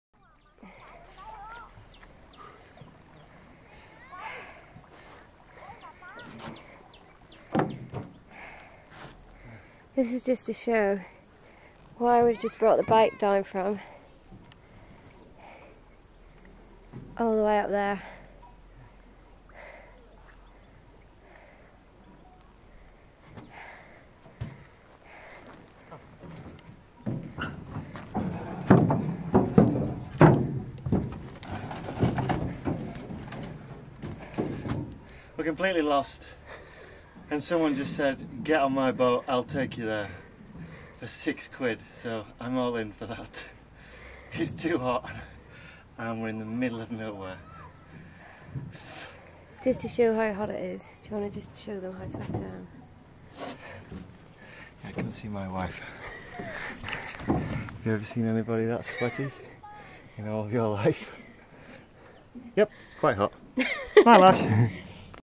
bike trail sweaty betty
Going off road on a Russian Minsk in Vietnam.